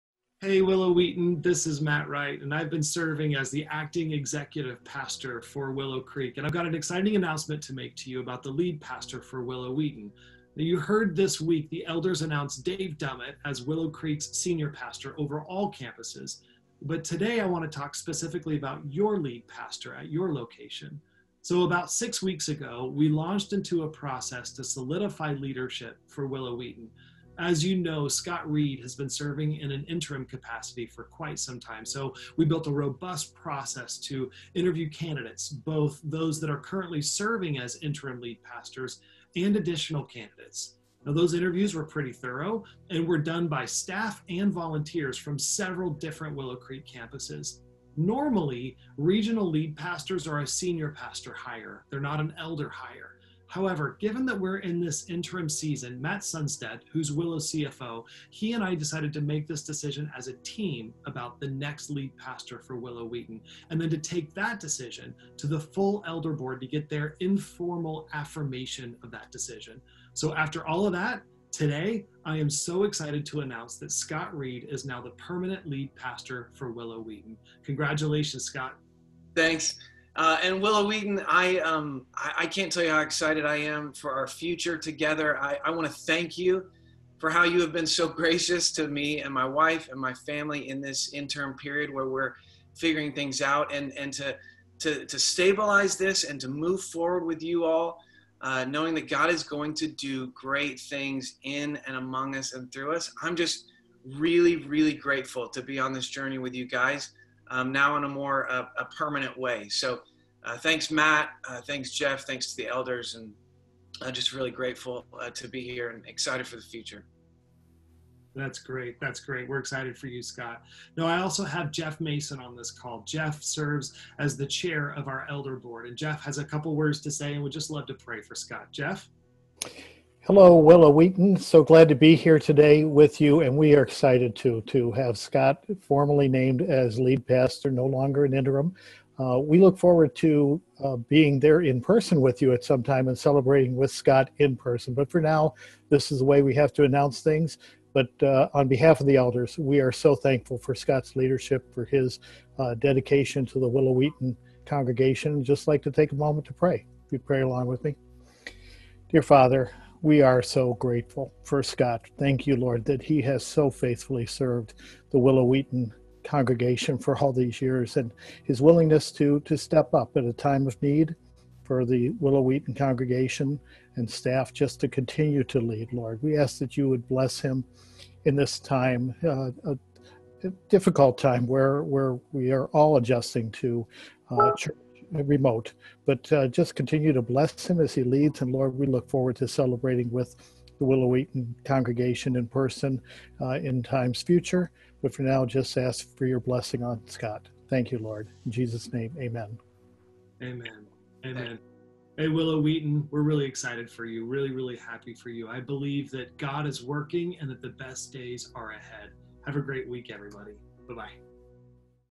Willow Wheaton Lead Pastor Announcement
Today, we are excited to inform you that Scott Reed has been named the permanent lead pastor for the Willow Wheaton campus!

Please join us in praying for Scott and his family.  It is with hope that we look forward to the exciting days that God has in store for our church!